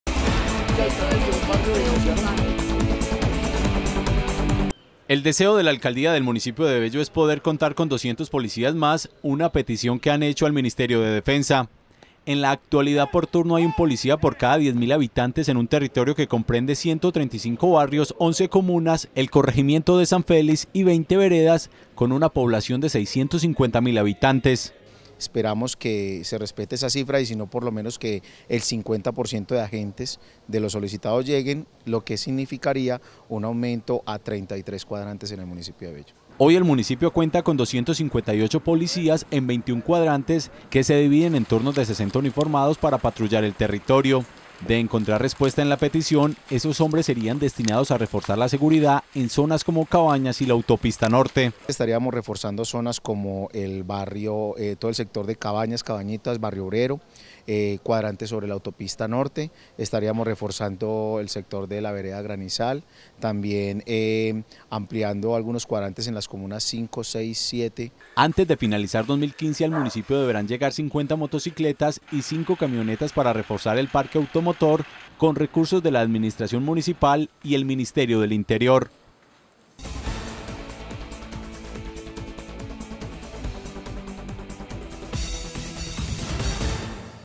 Por Cada 10 Mil Habitantes Hay un Policía En Bello | Teleantioquia Noticias
La administración municipal de Bello esta pidiendo un incremento en el número de policías debido ante el incremento de algunos delitos. En ese municipio por turno hay 60 uniformados para 650.000 habitantes.